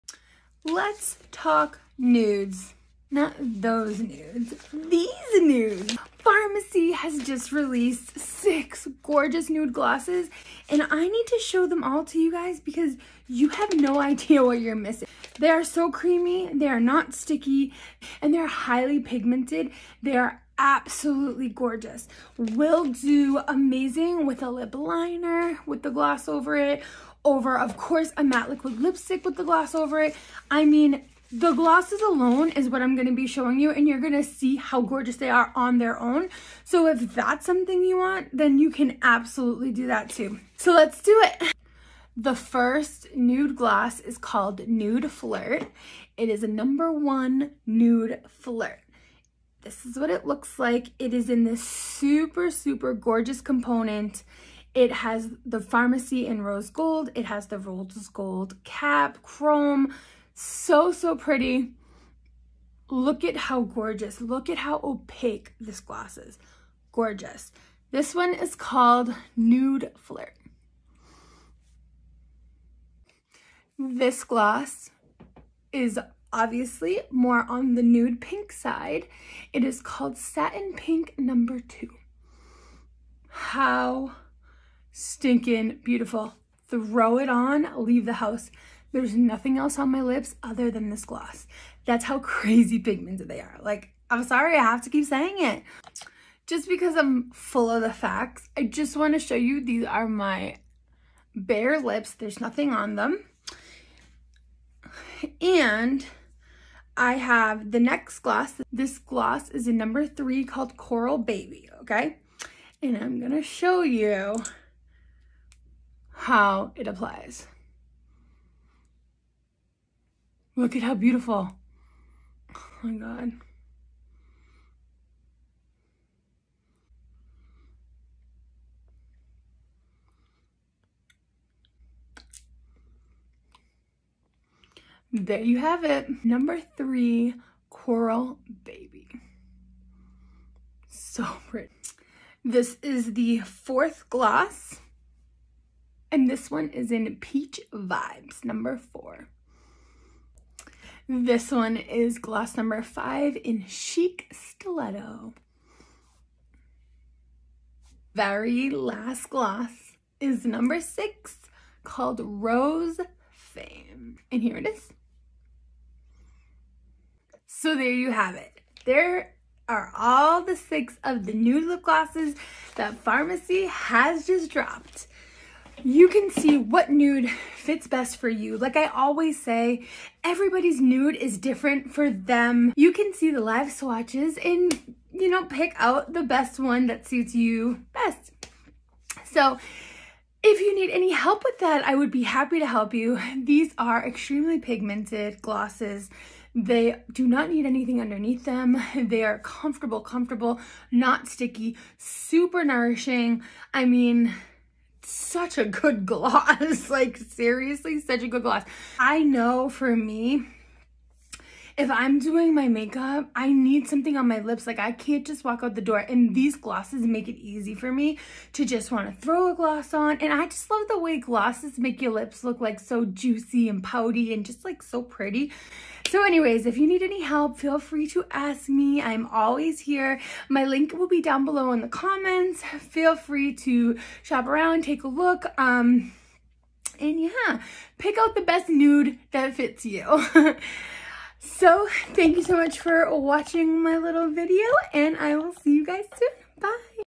NUDE GLOSSES | FARMASI
Farmasi has released nude glosses ans there so gorgeous!!!
What is your favorite one!!?

Join me for $19.99!!
-50% commissions from day one on all sales no downline needed!